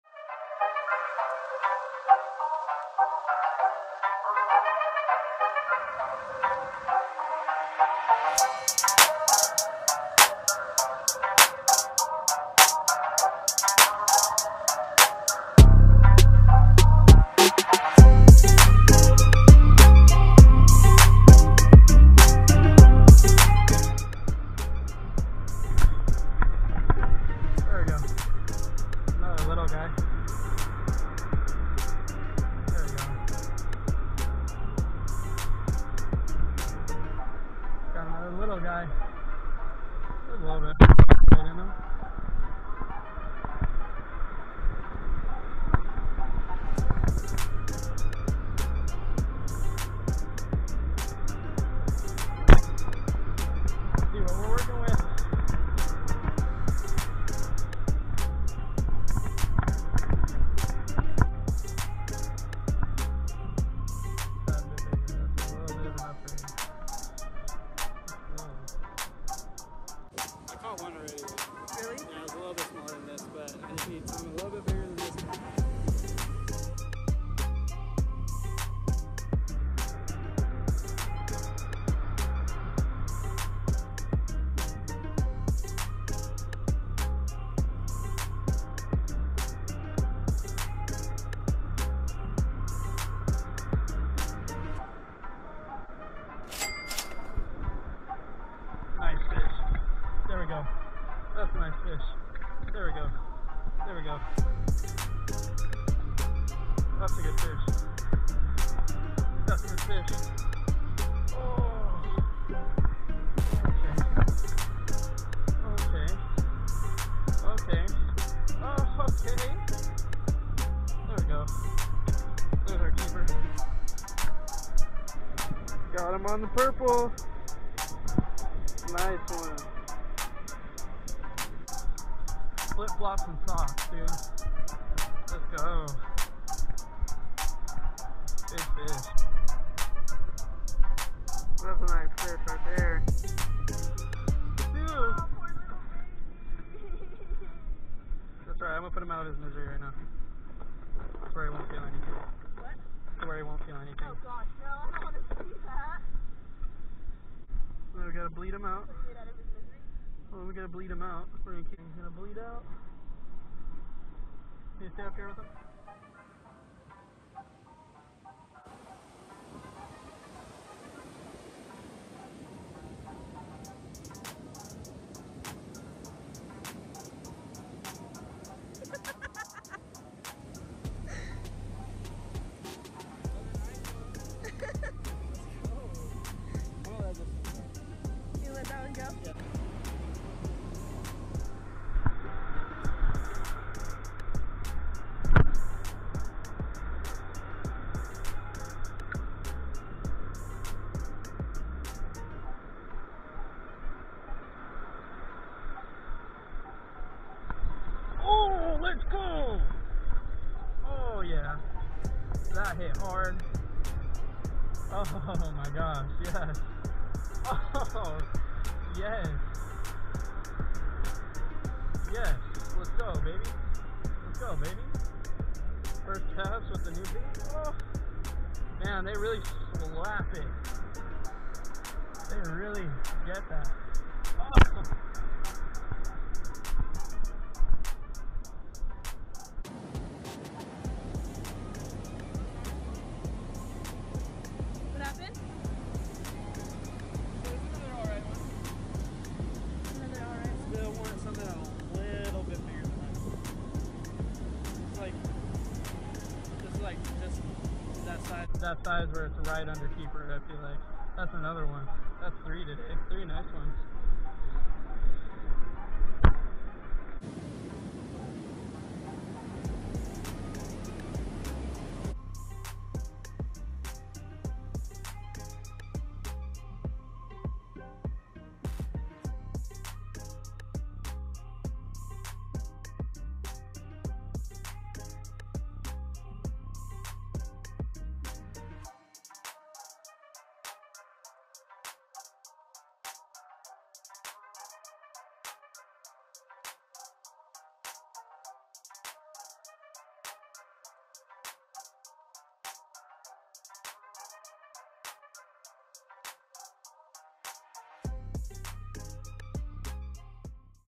YOU NEED this bait!! | RIPPEDEM’2022
Testing out “blurple” from flip flop n socks
Carolina rig
Size 2 hook
Instagram Rippem2022 Rolands.shop